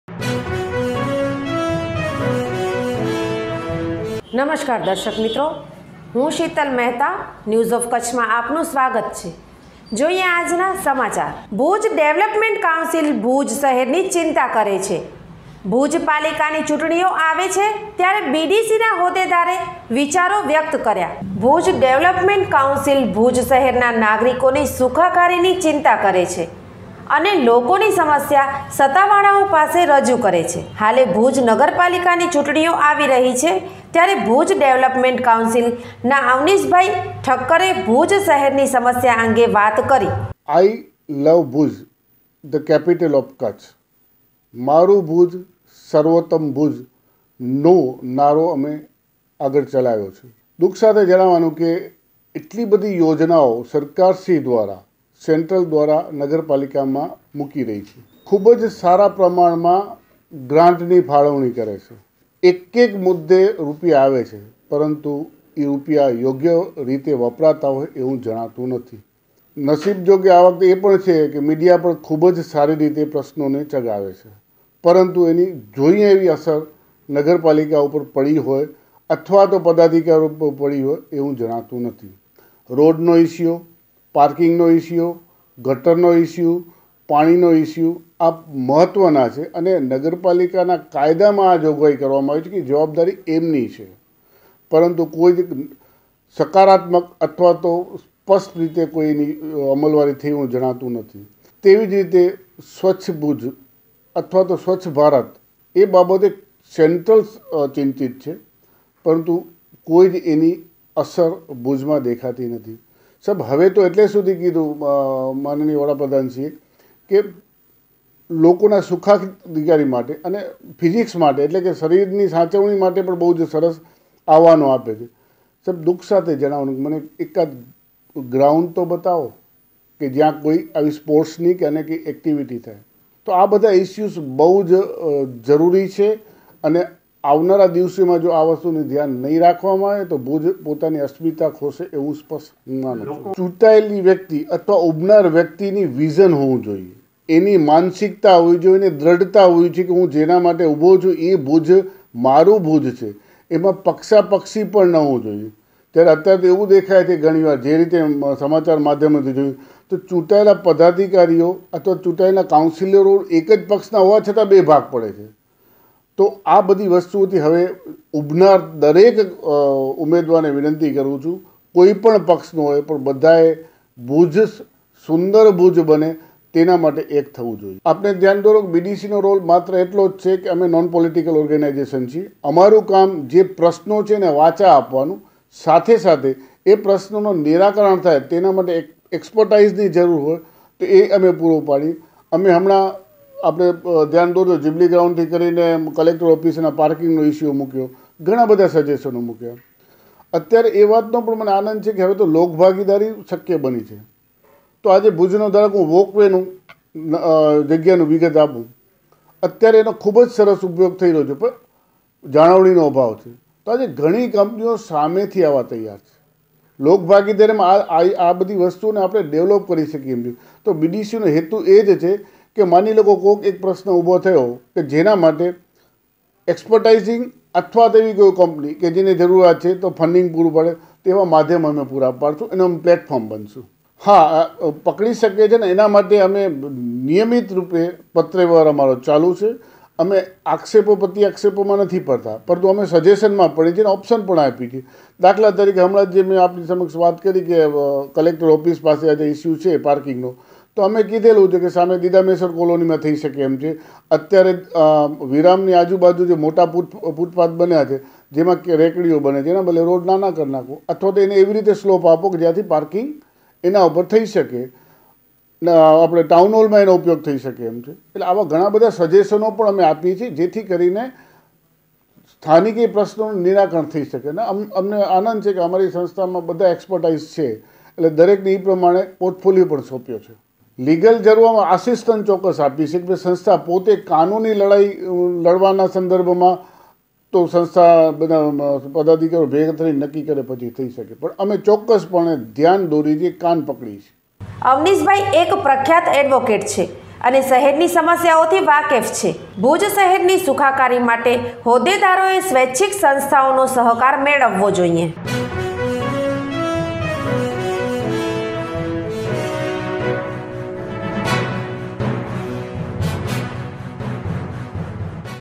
ભુજ ડેવલપમેન્ટ કાઉન્સિલ દ્વારા લોકોના સુખાકારી માટે વાત કરાઈ,જુઓ શું કહે છે એડવોકેટ અવનીશ ઠક્કર
ભુજ ડેવલપમેન્ટ કાઉન્સિલ દ્વારા  ભુજ ના લોકોના સુખાકારી માટે વાત કરાઈ,જુઓ શું કહે છે એડવોકેટ  અવનીશ ઠક્કર